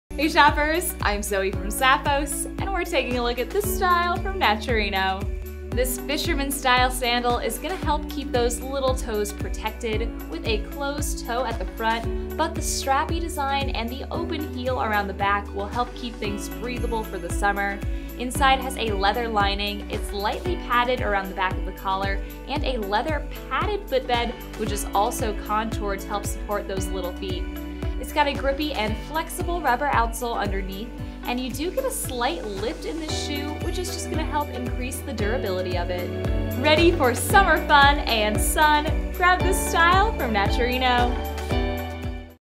Naturino Ziggy SS20 (Toddler/Little Kid) SKU: 9388887
Naturino Size Chart
Perfect for warm and sunny afternoon strolls, the sweet Naturino&reg; Ziggy SS20 fisherman sandal is sure to keep your little lady comfortable. Crafted from a metallic leather upper with a hook-and-loop closure strap for a secure fit.
Padded collar for all-day comfort.
Round, closed-toe silhouette.
Lightly padded leather lining and footbed.
Flexible, non-slip rubber outsole for durable wear.
Imported.

Product measurements were taken using size 24 (US 8.5 Toddler), width M. Please note that measurements may vary by size.